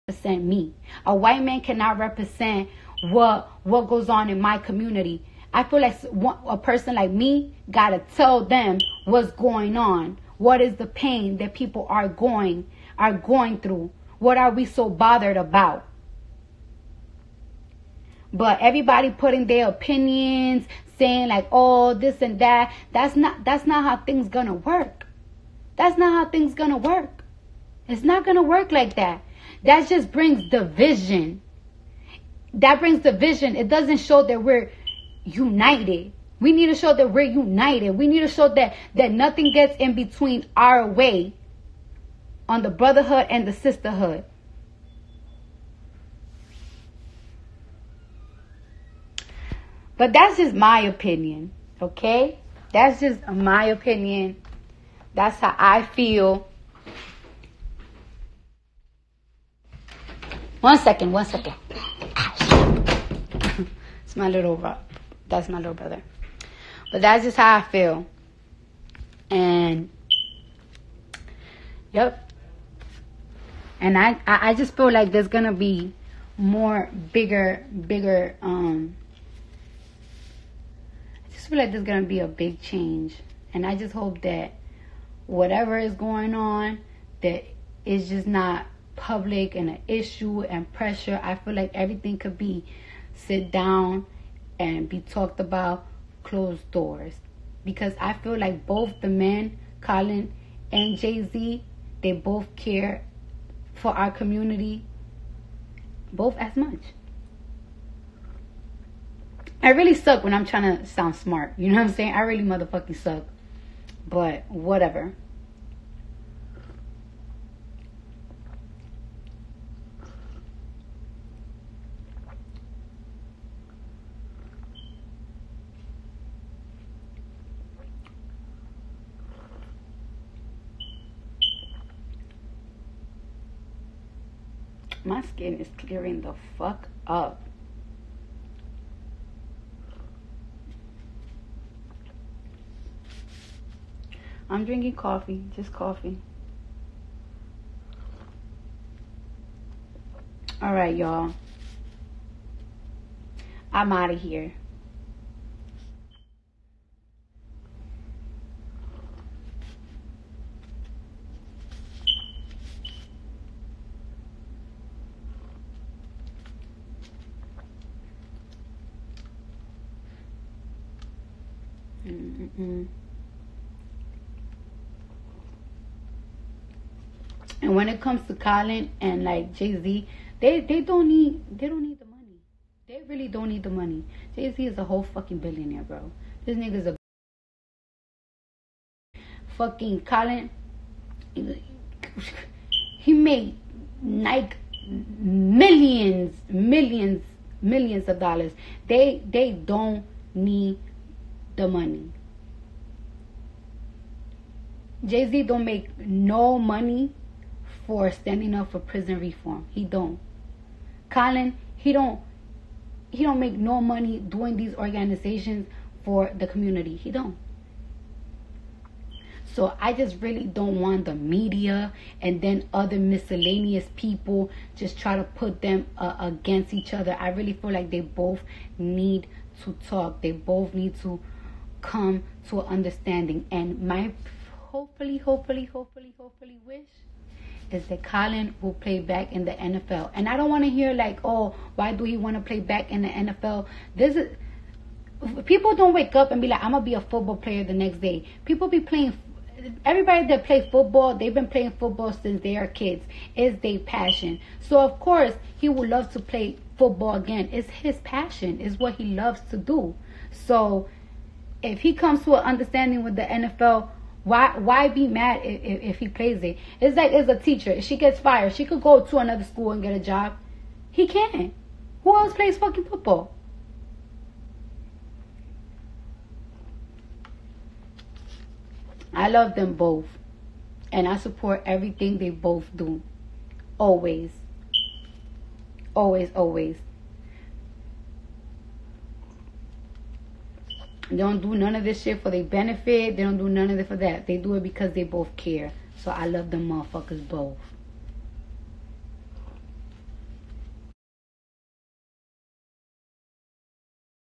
Cardi talks about Jay Z &Colin
Cardi B love her Ig talks about stuff going on today